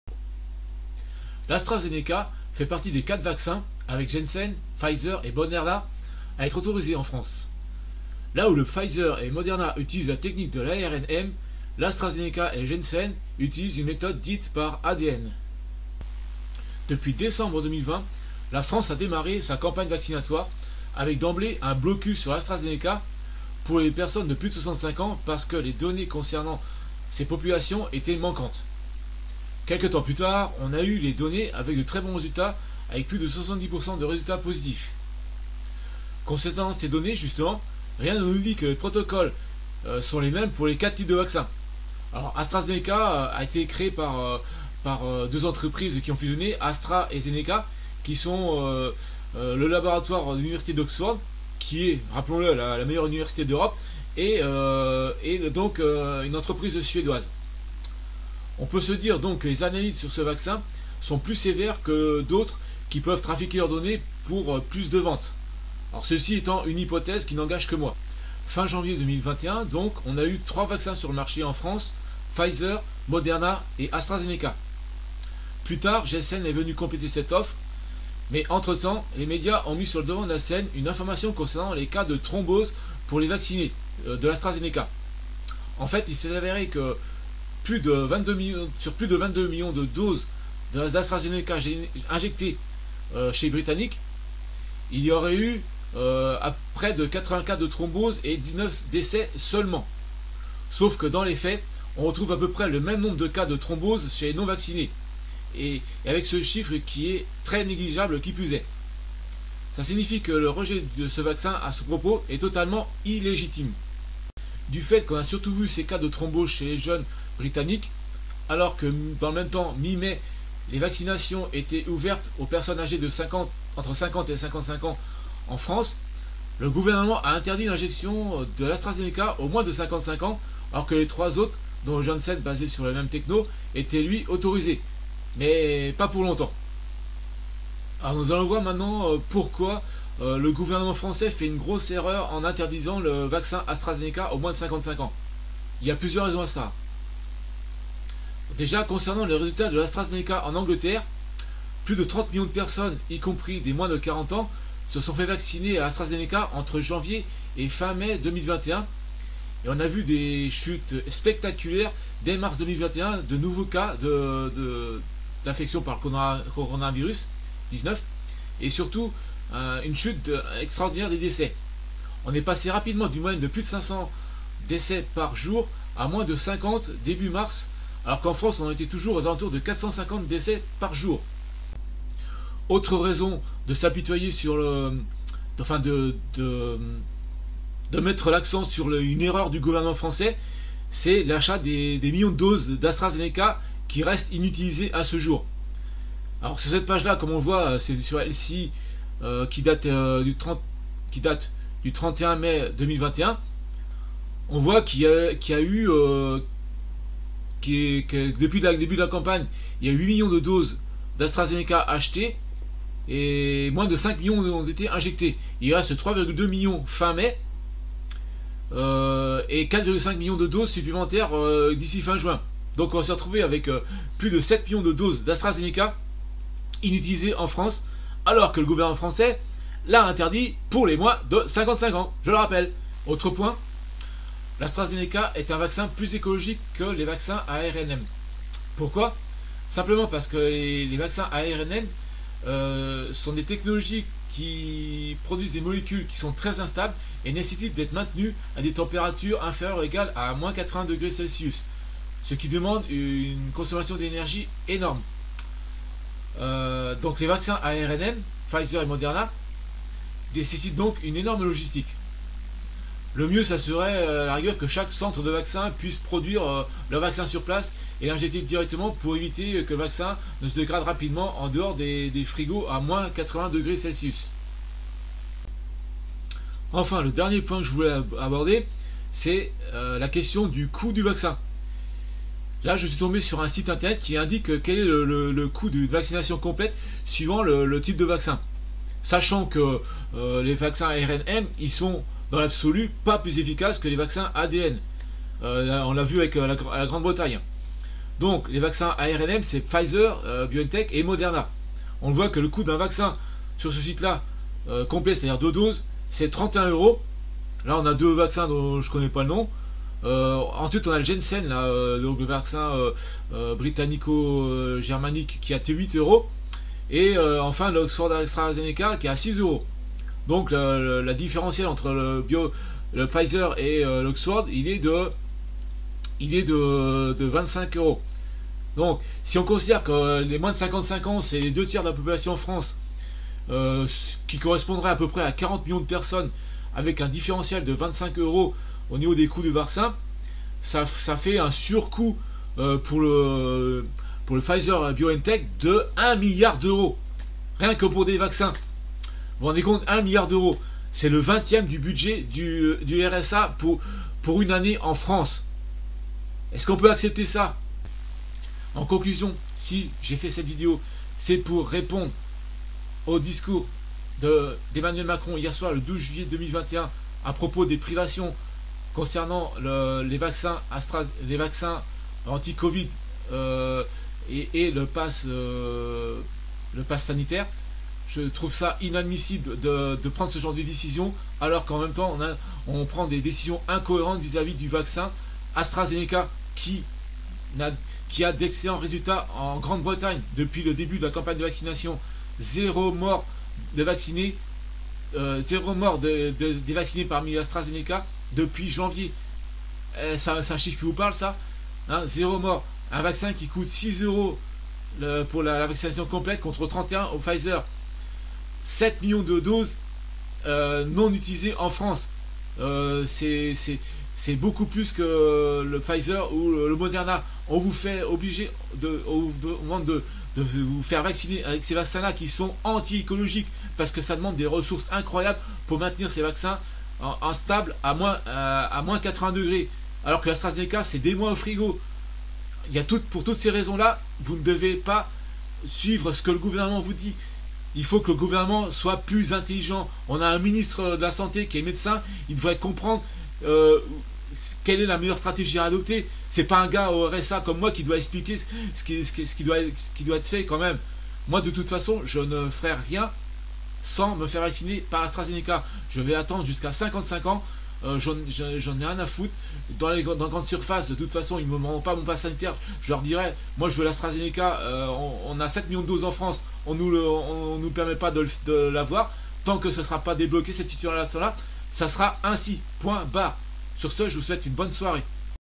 Vaccin Astrazenecca en France : un rejet injuste et illégal
L'incroyable gachis de la France vis à vis du vaccin AStraZenneca.